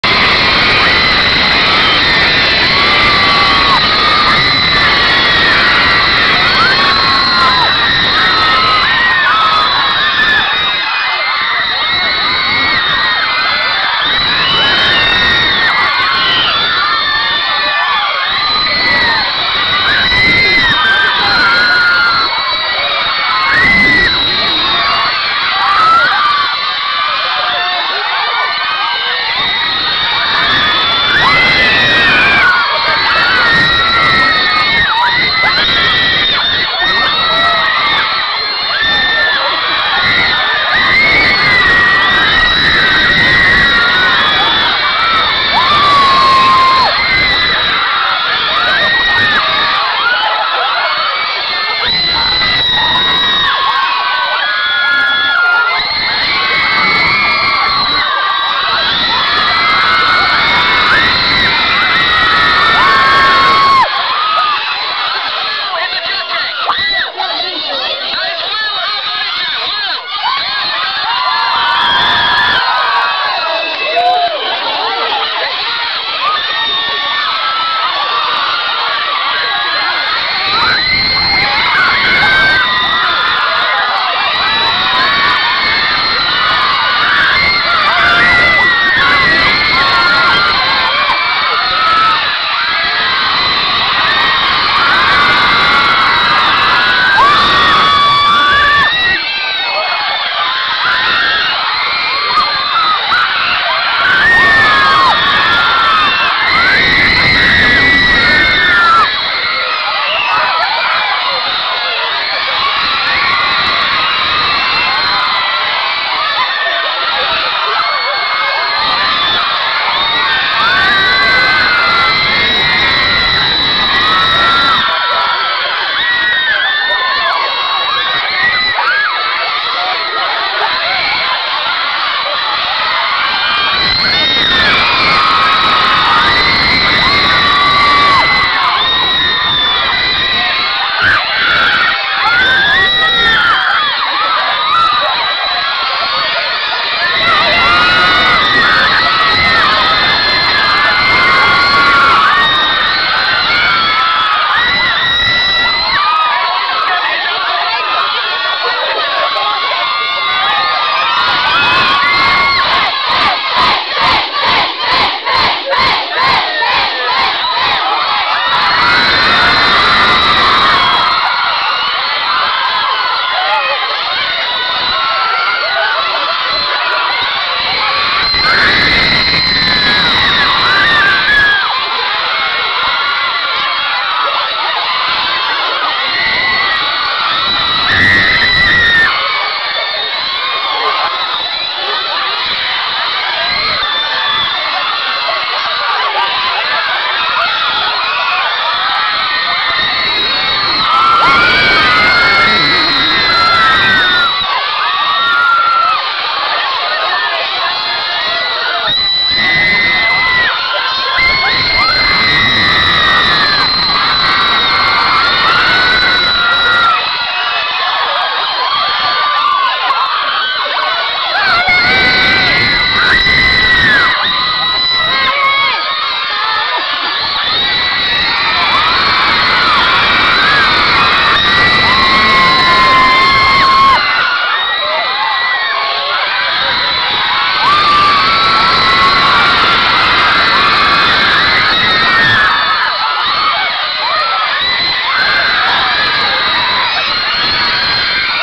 One Direction Radio Tour august 16th 2011
One Direction on the balcany outside the radio station :)